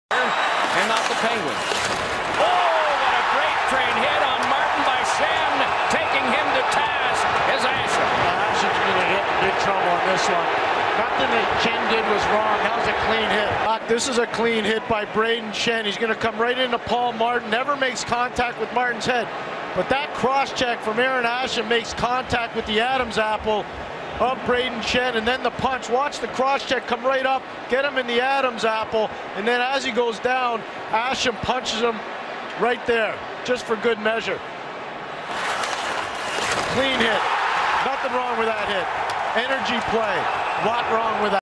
Brayden Schenn hits Paul Martin Round 1 Game 3 Flyers vs Penguins
Brayden Schenn Lays out Paul Martin, then gets a cheap shot from Arron Asham in Round 1 Game 3 of the Stanley Cup Playoffs